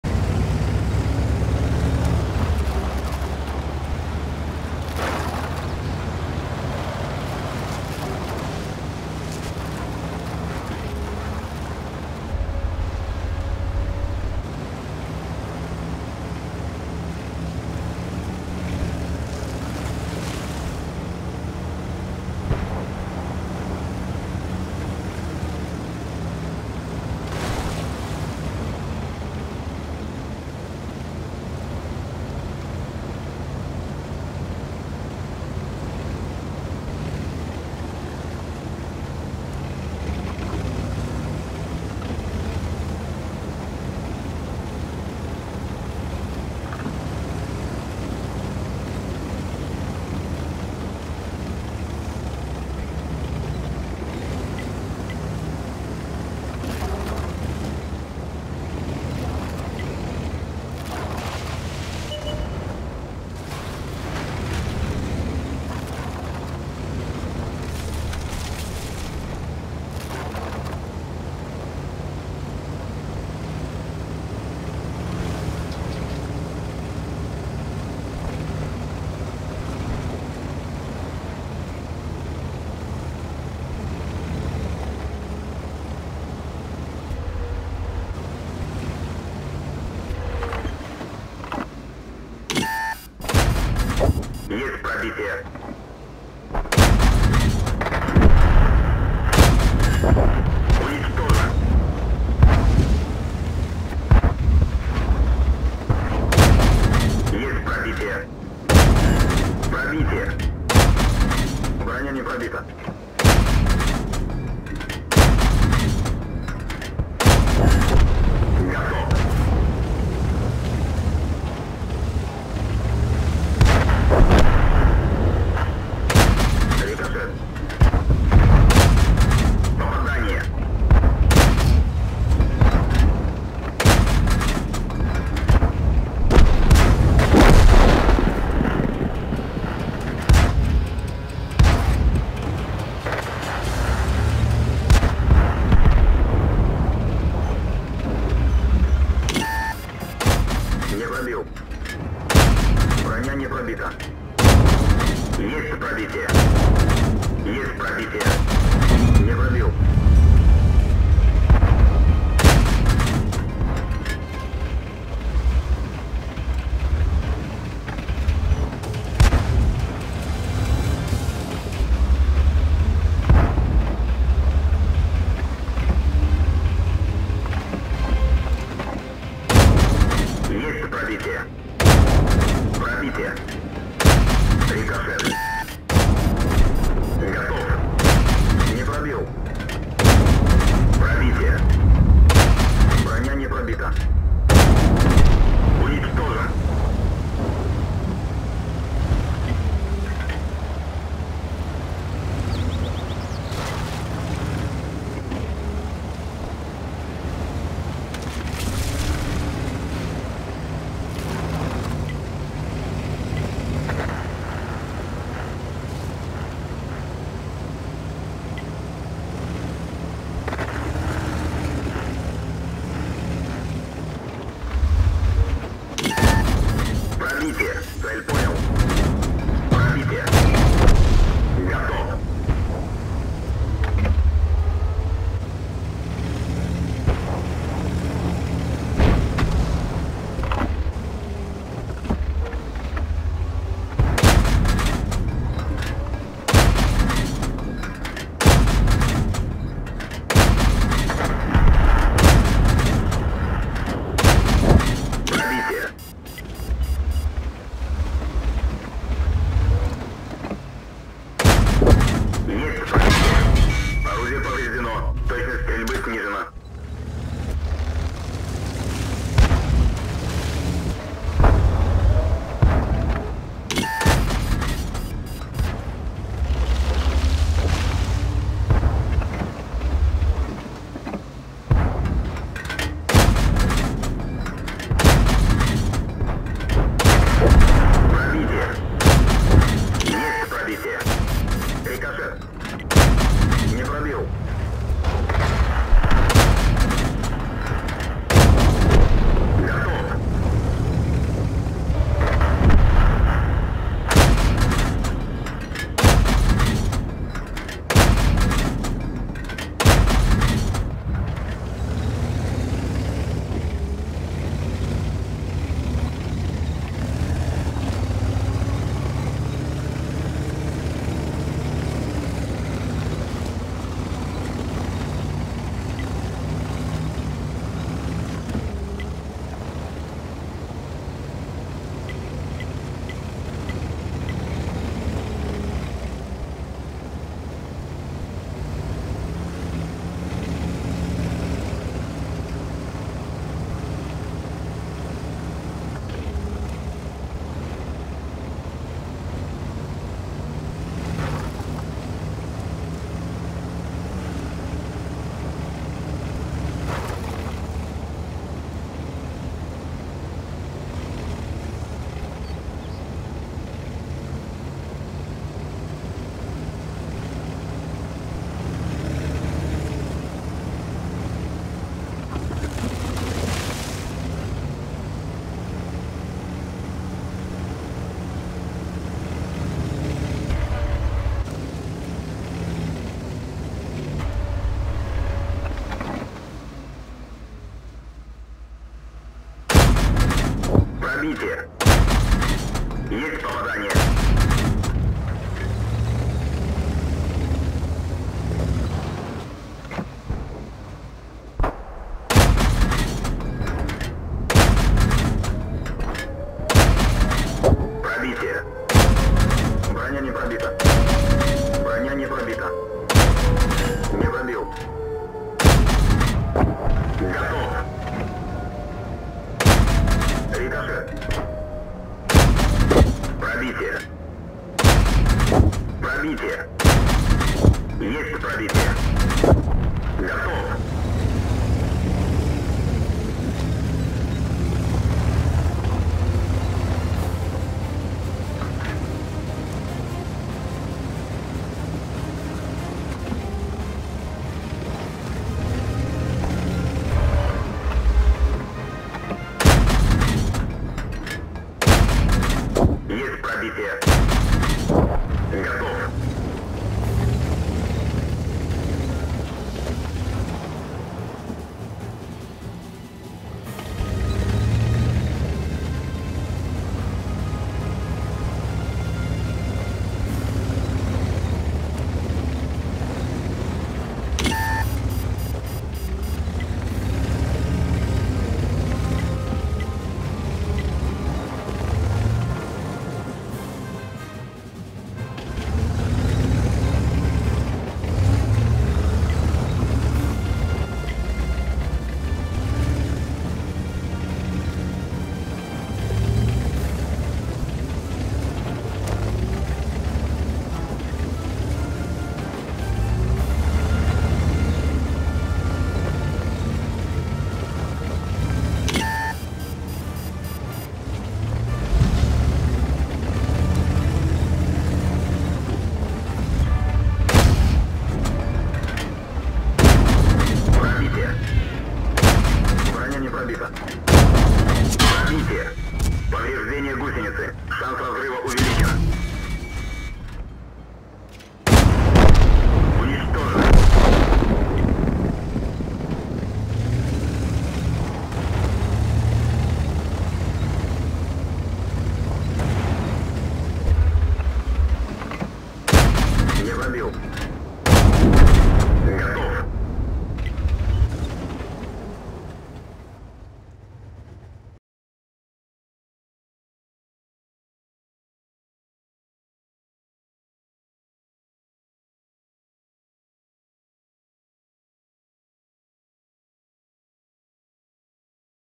Медаль Фадина, Пула, 11 фрагов на Т-34
Эпичная концовка, казалось уже нет шансов на победу, проигрыш по фрагам и по ХП, отличная игра союзного игрока на Т-34-85М и дело в шляпе.
P.S. после боя письма благодарности не только от союзной команды, но и от вражеской.